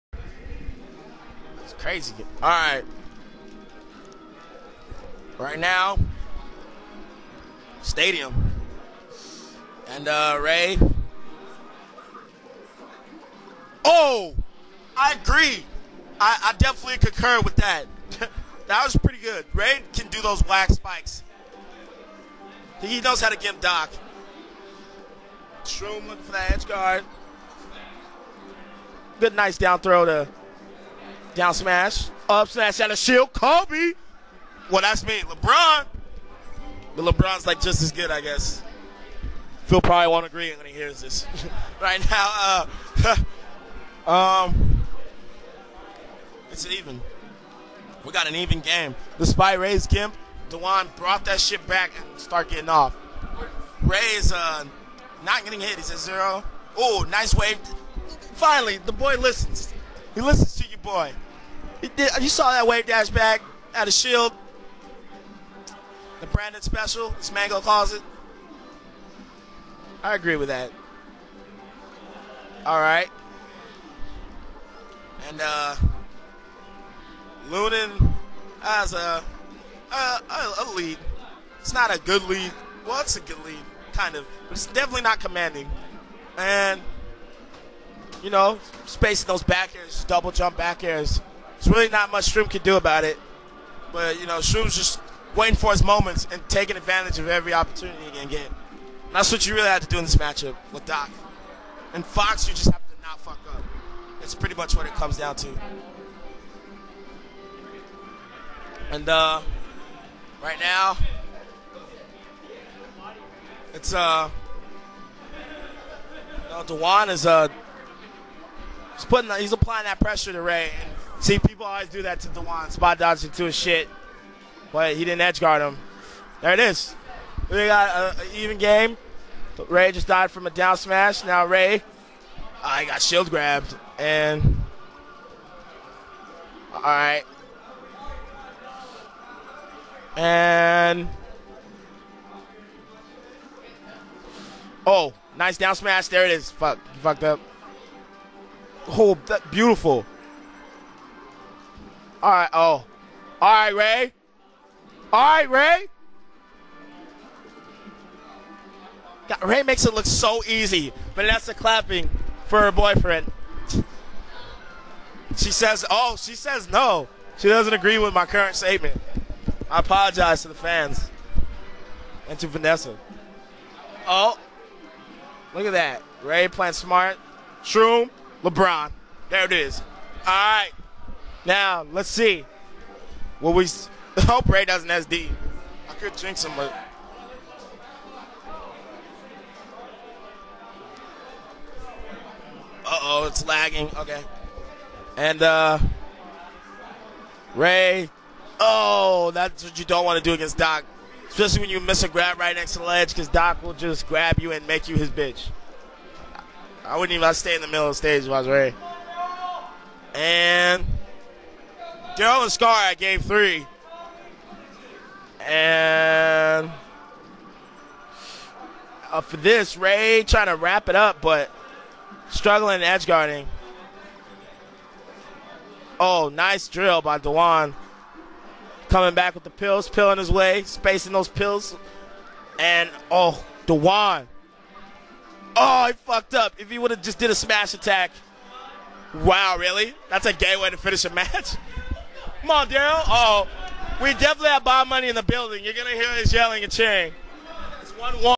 Lunin(Fox) vs Shroomed(Doc)2
CGC @ SFSU R00RFEST

01/09/10

Winners Semis(Best of 5)

Commentary By: HMW and Phil